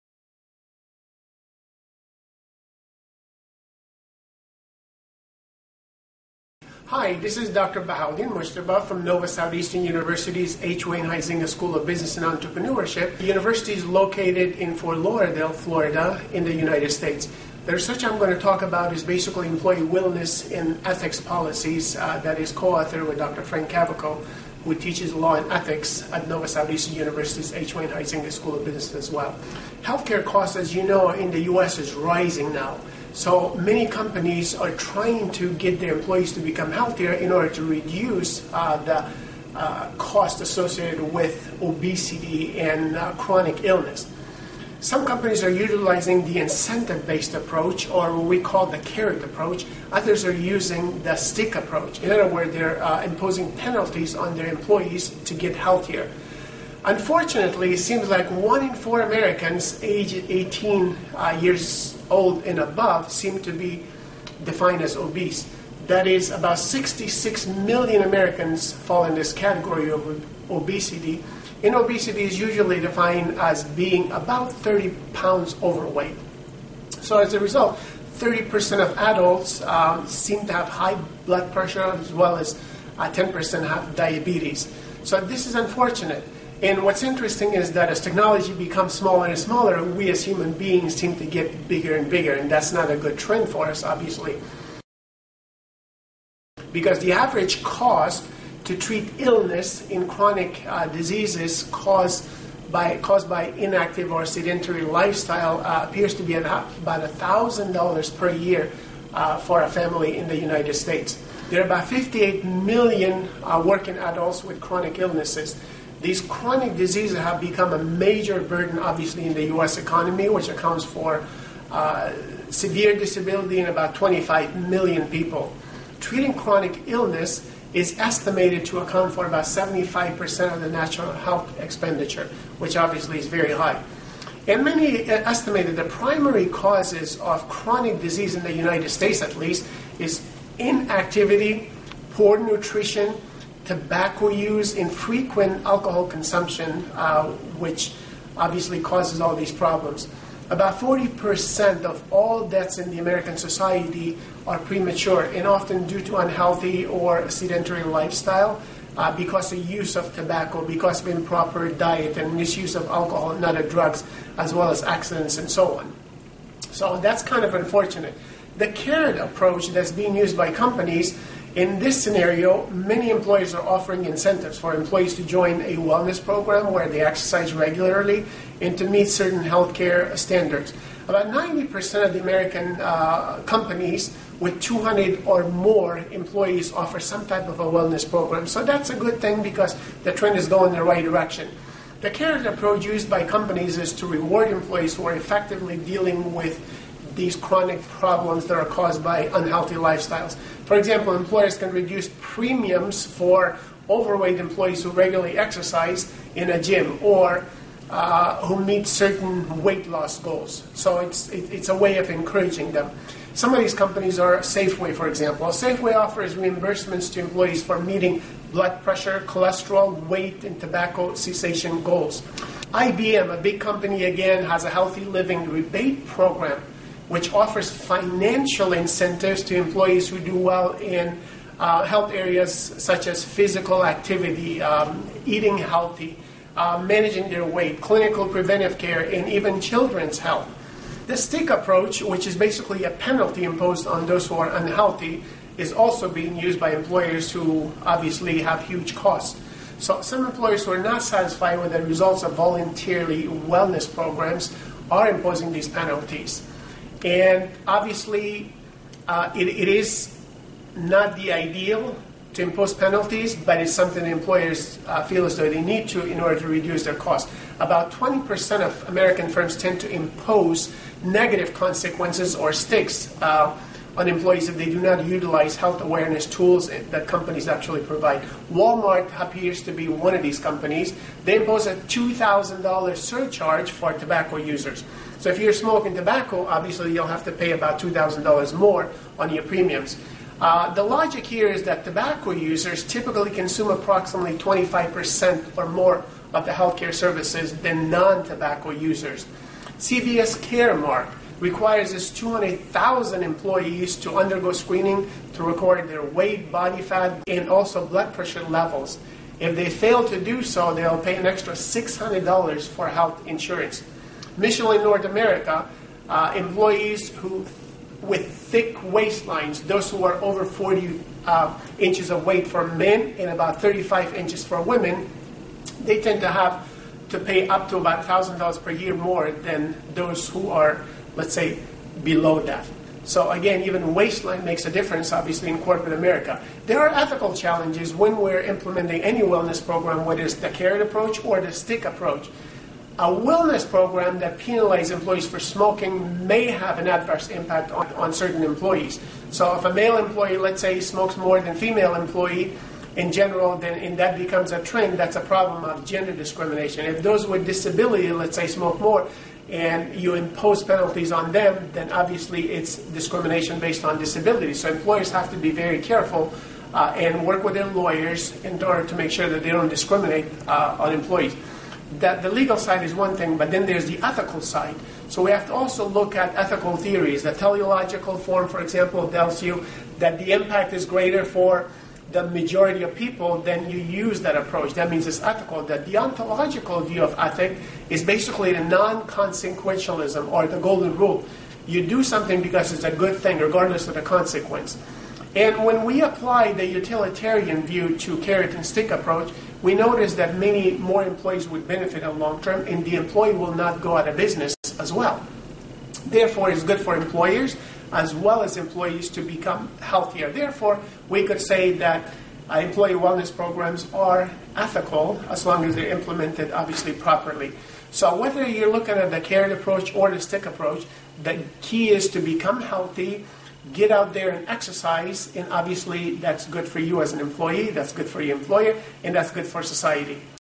Wellness Programs and Ethics Policies
Dr. Bahaudin Mujtaba from Nova Southeastern University in USA